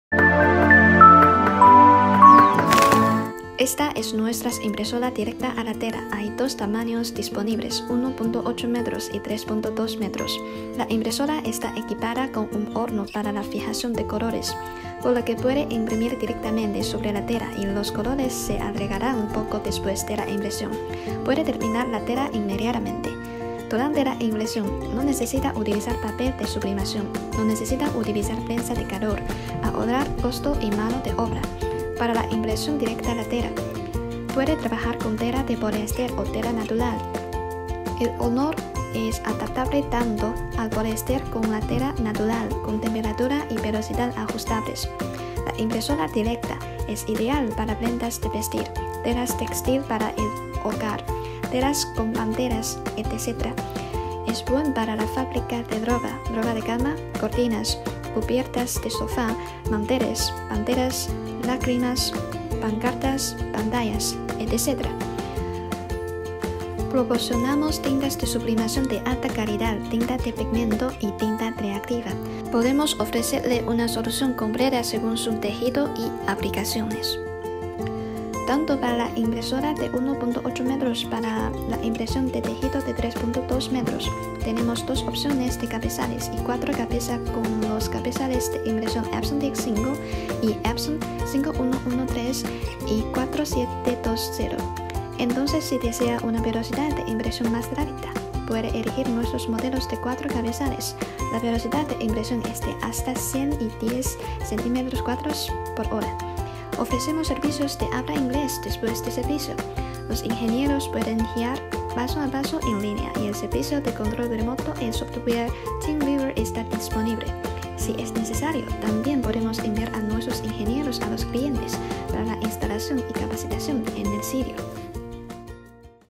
Sublimation Printer
1.8m width , with 2/4 pcs epson 4720 heads 140sqm per hour production .
print on transfer paper , use for all kinds of polyester fabric .